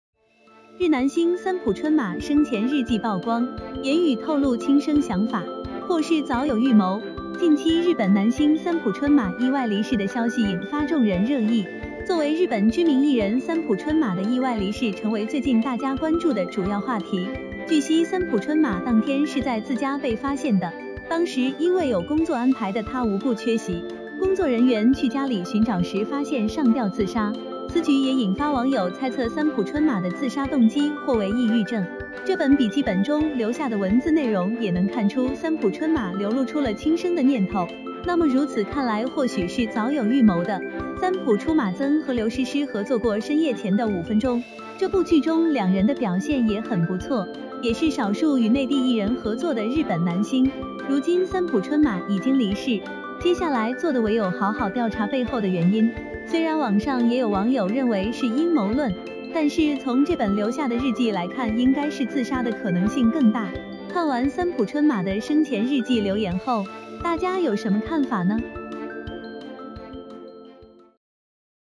日男星三浦春马生前日记曝光,言语透露轻生想法,或是早有预谋
➤Thanks收看！